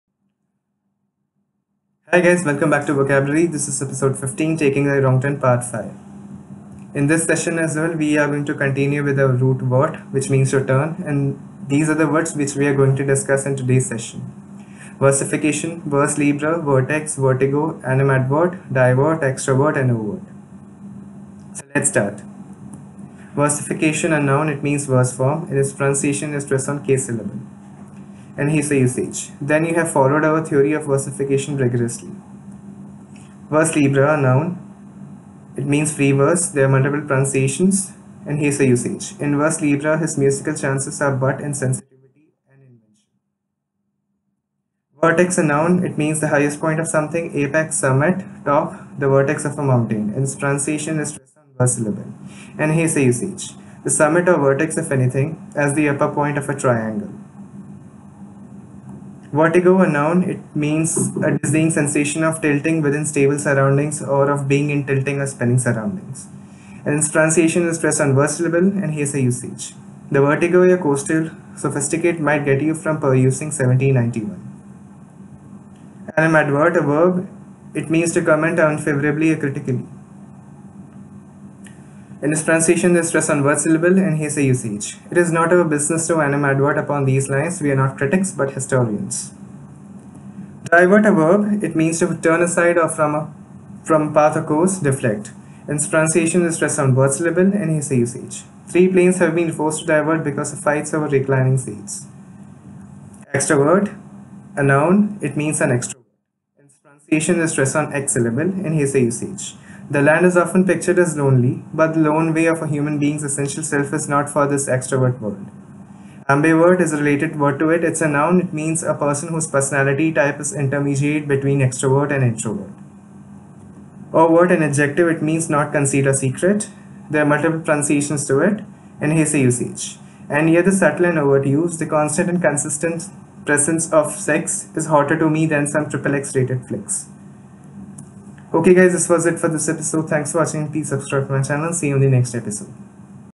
Vocabulary: Taking The Wrong Turn Part-5
Learn about "VERT" and its various derivatives.